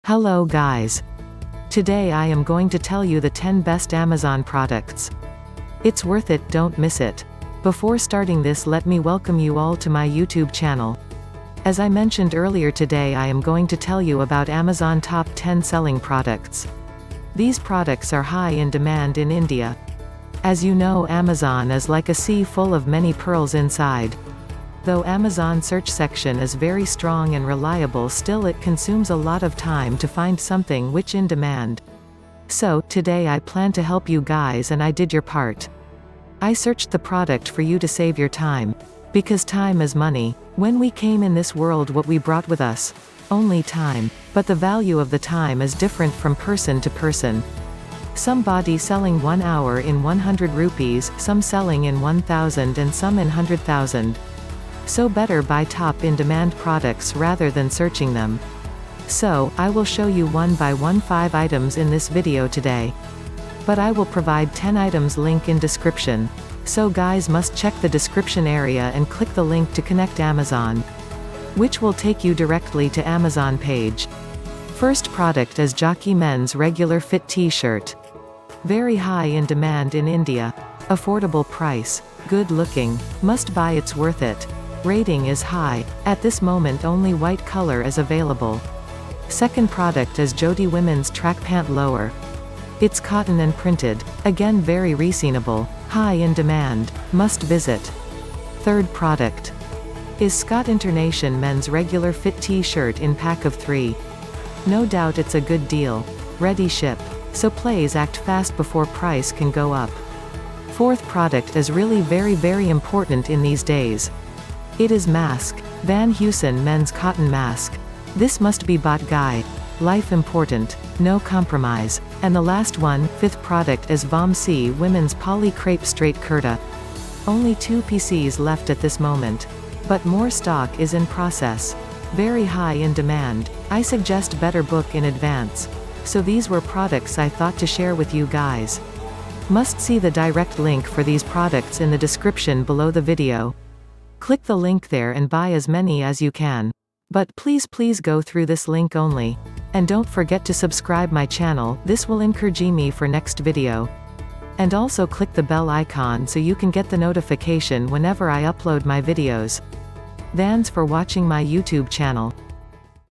10 Best Selling Clothing & Accessories on Amazon!
Description: As you know “Amazon” is like a sea which has many pearls inside. I found some for you. Click the links below and find it. We know that “Time is Money “, I saved yours so, you can spend in buying.

Product List:
1) Jockey Men's Regular Fit T-Shirt

2) jyoti Womens Track Pant Lower Cotton Printed Payjama

3) Scott International Men's Regular Fit T-Shirt (Pack of 3)
4) Van Heusen Men's Cotton Face Mask

5) Vaamsi Women's Poly Crepe straight Kurta (VPK1583Par_ Yellow_ Large)

6) Jockey Women's Cotton Hipster (Pack of 3) (Colors may vary)(color may vary)
7) Longies Men's Printed Cotton Boxers (Combo Pack of 3)

8) Jockey Women's Cotton Bikini (Pack of 3) (color may vary)

9) LUX VENUS Men's Cotton Vest (Pack of 6)

10) Jockey Women's Cotton Full Coverage Shaper Bra